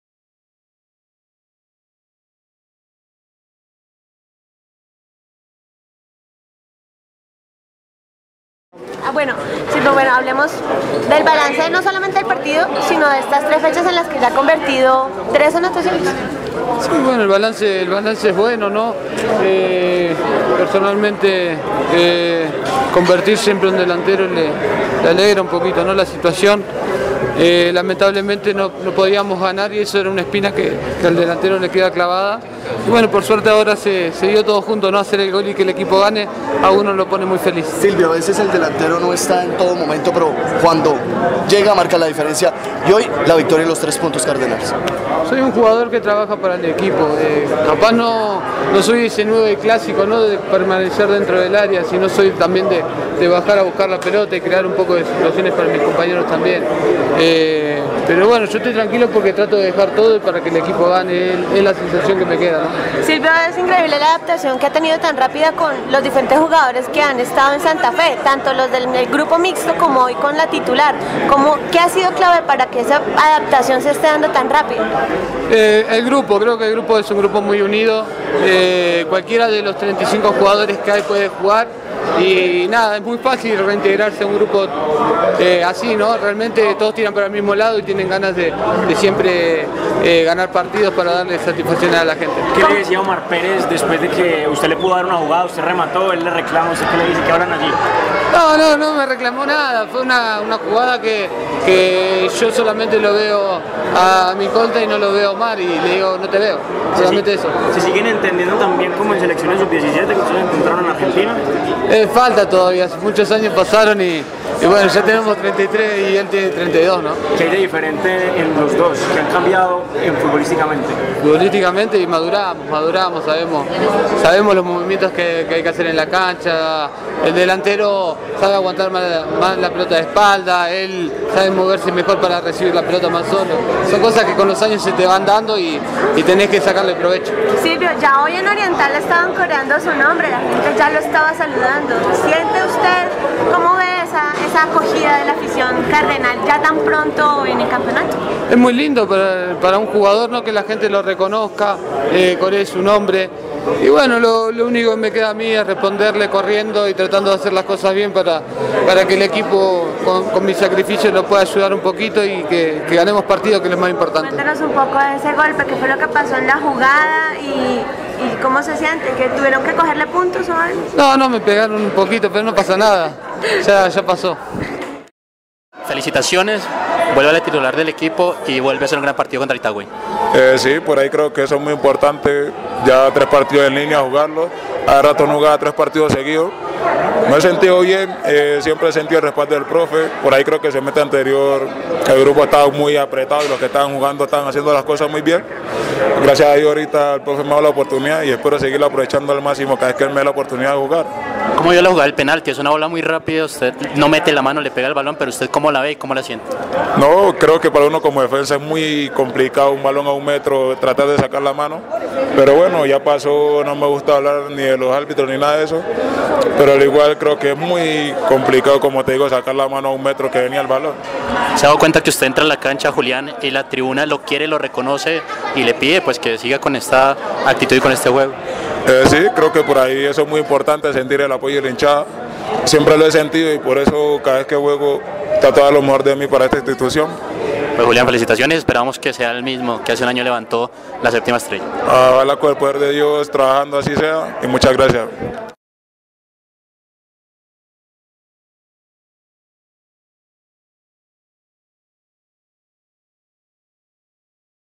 Reacciones Silvio González y Julián Quiñones ::: Independiente Santa Fe vs. Itagüí
Las palabras de Silvio González y Julián Quiñones luego de la victoria 2x1 de Independiente Santa Fe ante Itagüí.